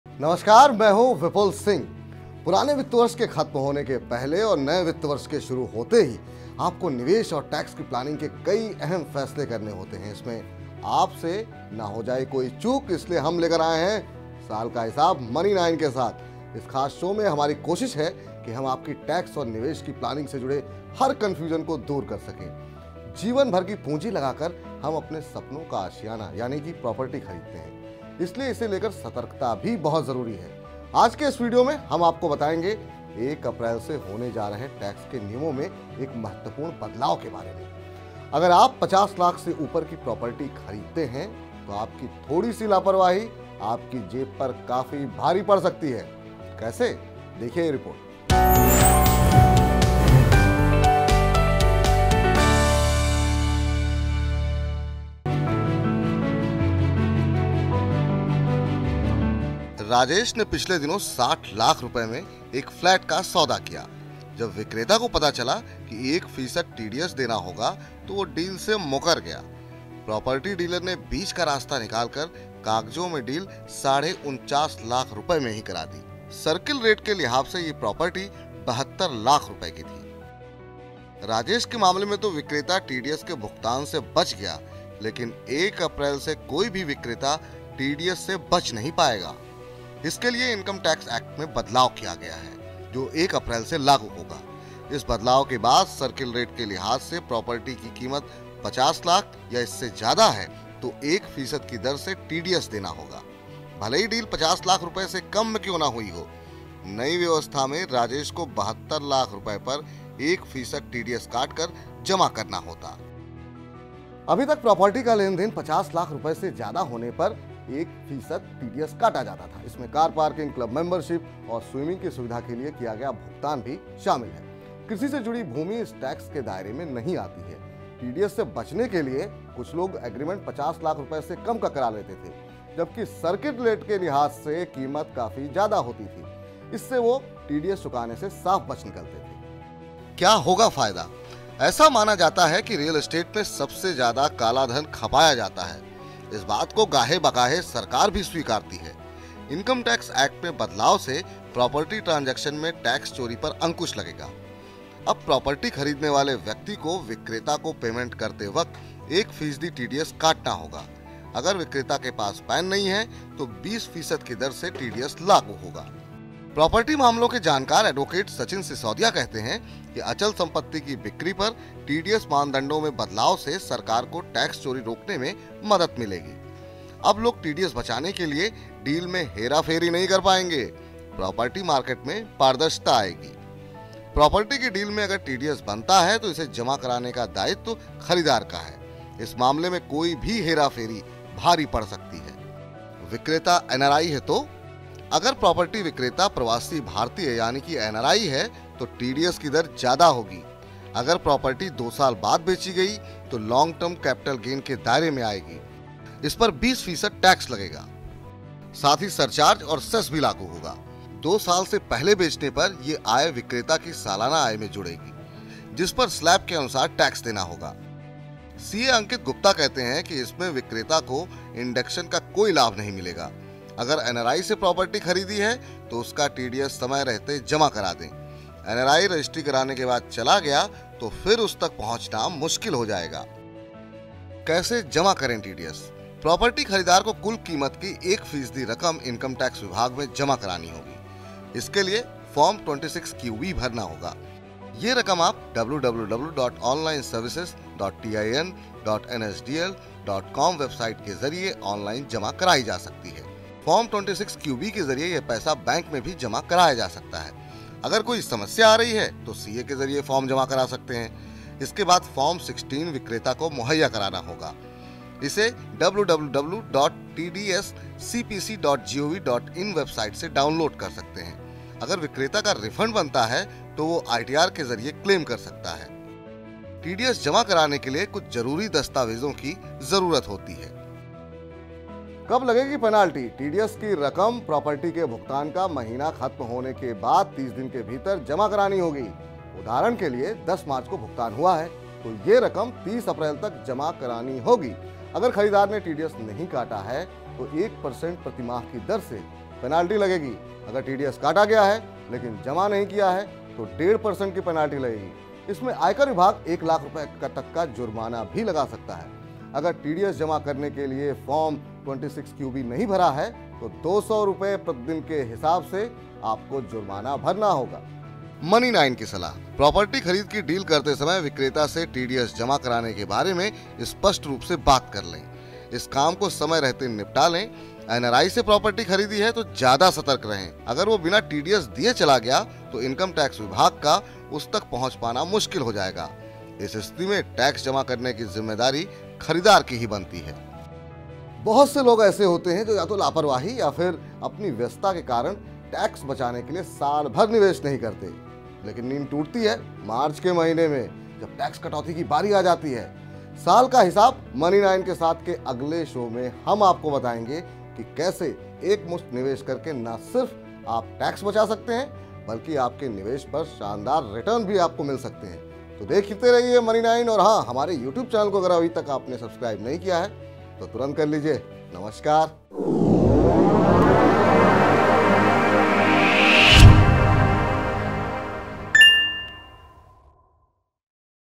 EP6: साल का हिसाब | Property खरीद रहे हैं तो पहले समझ लें TDS का पूरा गणित | Property TDS Rate
अगर कोई प्रॉपर्टी 50 लाख रुपए से अधिक की कीमत में बेची जा रही है तो इस पर एक फीसद टीडीएस लगता है. इस टैक्स को विक्रेता वहन करेगा, जबकि जमा कराने का दायित्व खरीदार है. इस बारे में एक अप्रैल से बड़ा बदलाव होने जा रहा है. जानिए इस वीडियो में-